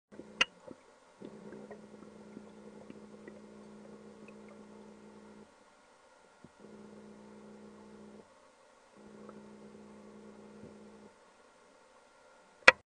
Represented Footage From Wet Waders
A manatee slowly swims down to the riverbed.